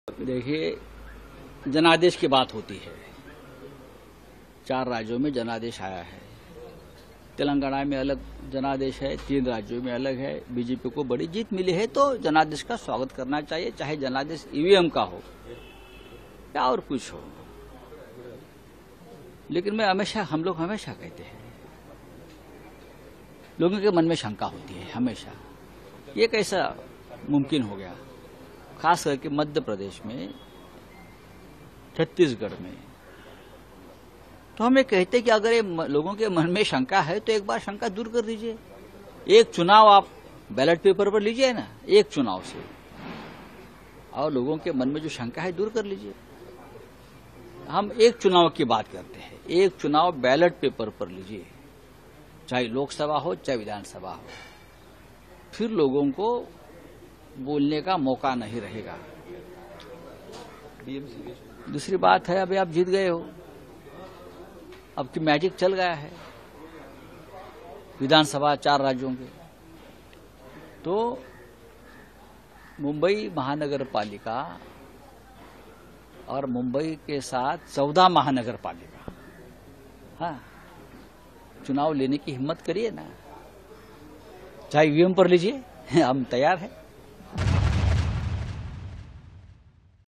Sanjay Raut ने उठाए चुनावी नतीजों पर सवाल, कहा- 'लोगों के मन में संदेह है कि यह कैसे संभव है'
विधानसभा चुनाव (Assembly Election) के नतीजों पर अलग अलग राजनीतिक दलों की प्रतिक्रिया सामने आ रही है. इसी बीच शिवसेना (यूबीटी) सांसद संजय राउत (Shivsena MP Sanjay Raut) का कहना है, जनादेश का स्वागत किया जाना चाहिए।" लेकिन हम हमेशा कहते हैं- लोगों के मन में संदेह है कि यह कैसे संभव है, खासकर मध्य प्रदेश (Madhya Pradesh) और छत्तीसगढ़ (Chattisgarh) में। इसलिए, हम कहते हैं कि अगर संदेह है, तो इसे दूर कर लें। एक चुनाव बैलेट पेपर (Ballet Paper) पर हो। बस एक चुनाव - और संदेह को दूर कर दें..."